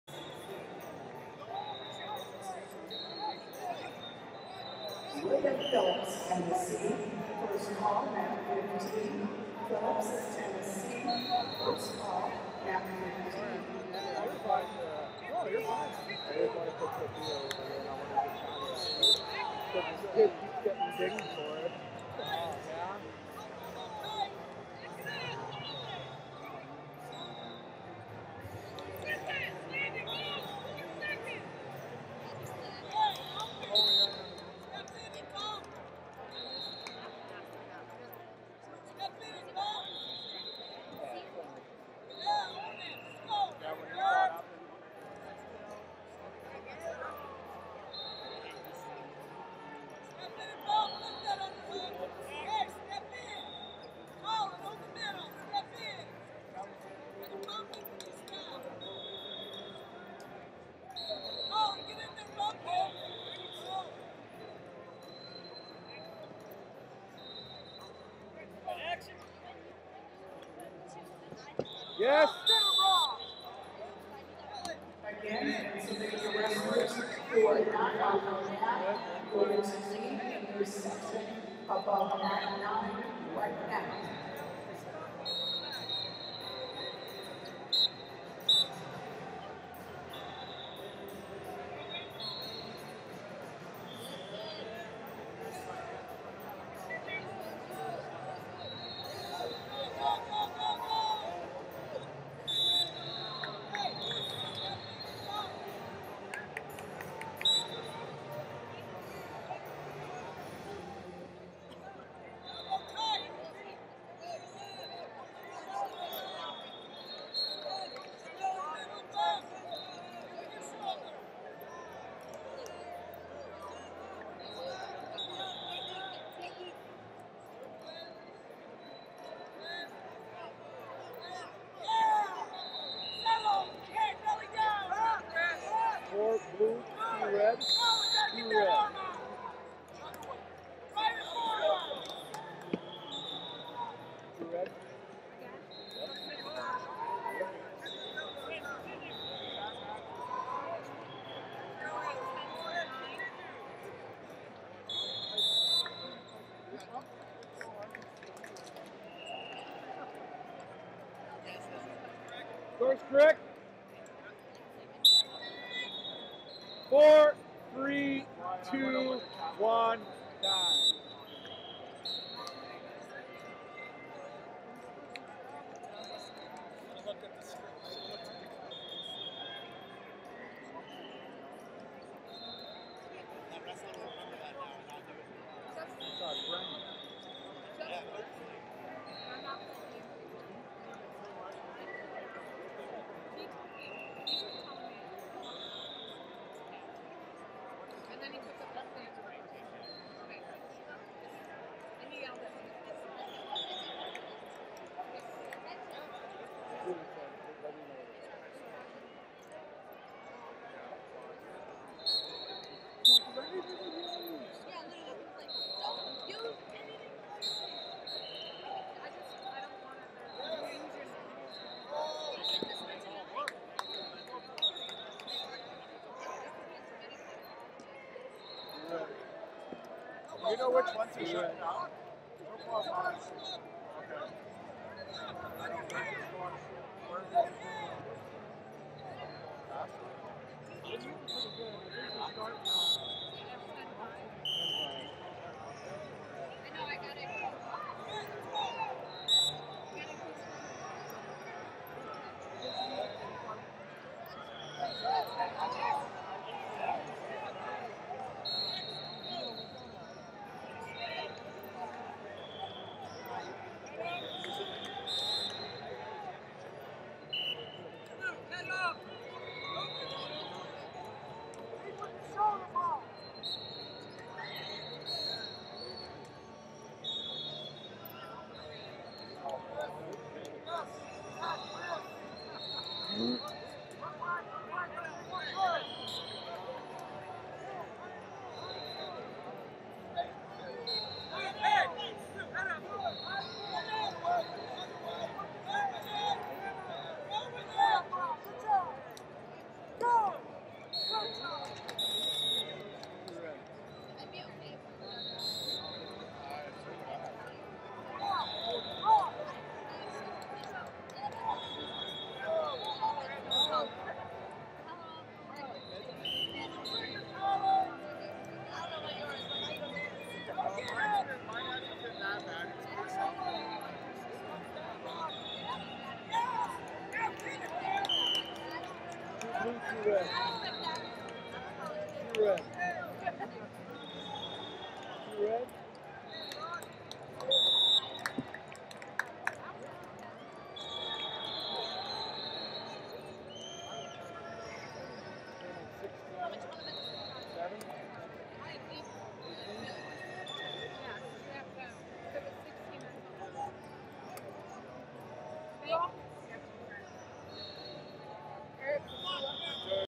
94 Lbs Consi Of 8 #1 - Brock Johnson, Kansas Vs Colin George, Colorado C37d
2022 USMC/USAW Junior And 16U National Championships - 94 Lbs Consi Of 8 #1 - Brock Johnson, Kansas Vs Colin George, Colorado C37d